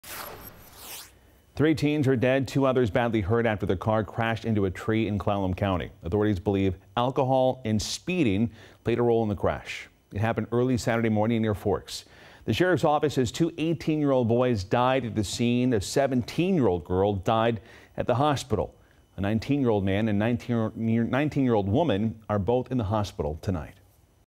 3 teens dead, 2 critically injured after vehicle collides with tree near Forks
Clallam County Sheriff officials said alcohol and speeding are suspected to be contributing factors in the deadly collision.